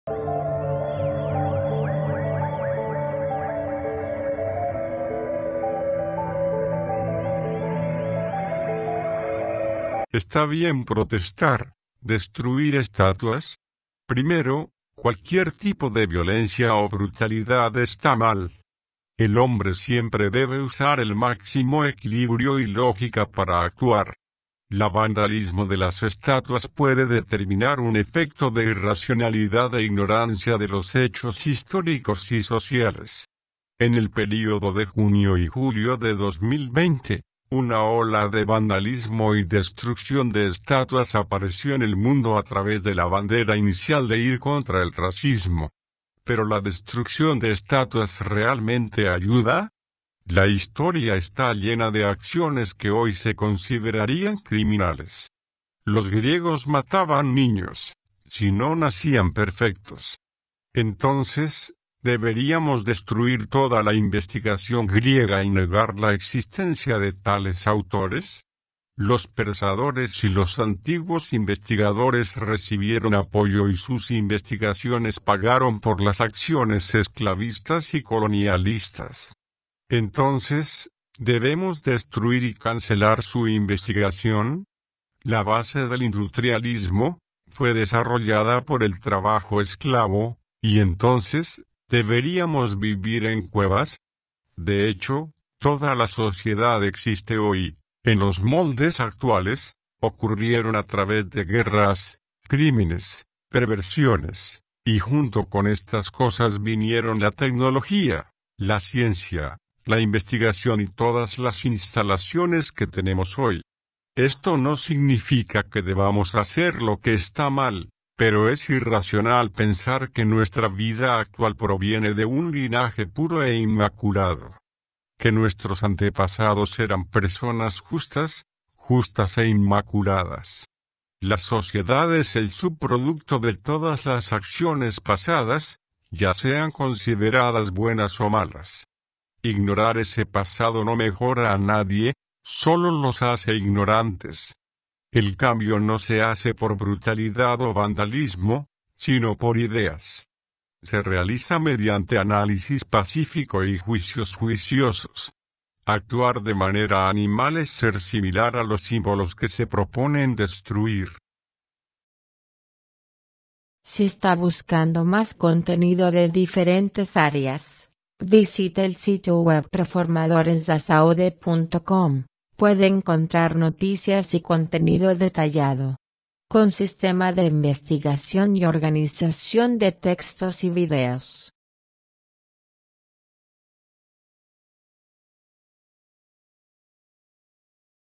¿Está bien protestar, destruir estatuas? NC5-E150-E
Primero, cualquier tipo de violencia o brutalidad está mal. El hombre siempre debe usar el máximo equilibrio y lógica para actuar.
La vandalismo de las estatuas puede determinar un efecto de irracionalidad e ignorancia de los hechos históricos y sociales.
En el período de junio y julio de 2020, una ola de vandalismo y destrucción de estatuas apareció en el mundo a través de la bandera inicial de ir contra el racismo.
¿Pero la destrucción de estatuas realmente ayuda?
La historia está llena de acciones que hoy se considerarían criminales. Los griegos mataban niños, si no nacían perfectos. Entonces, ¿deberíamos destruir toda la investigación griega y negar la existencia de tales autores?
Los pensadores y los antiguos investigadores recibieron apoyo y sus investigaciones pagaron por las acciones esclavistas y colonialistas. Entonces, ¿debemos destruir y cancelar su investigación?
La base del indutrialismo, fue desarrollada por el trabajo esclavo, y entonces ¿deberíamos vivir en cuevas?
De hecho, toda la sociedad existe hoy, en los moldes actuales, ocurrieron a través de guerras, crímenes, perversiones, y junto con estas cosas vinieron la tecnología, la ciencia, la investigación y todas las instalaciones que tenemos hoy.
Esto no significa que debamos hacer lo que está mal, pero es irracional pensar que nuestra vida actual proviene de un linaje puro e inmaculado. Que nuestros antepasados ​​eran personas justas, justas e inmaculadas.
La sociedad es el subproducto de todas las acciones pasadas, ya sean consideradas buenas o malas. Ignorar ese pasado no mejora a nadie, solo los hace ignorantes.
El cambio no se hace por brutalidad o vandalismo, sino por ideas. Se realiza mediante análisis pacífico y juicios juiciosos. Actuar de manera animal es ser similar a los símbolos que se proponen destruir.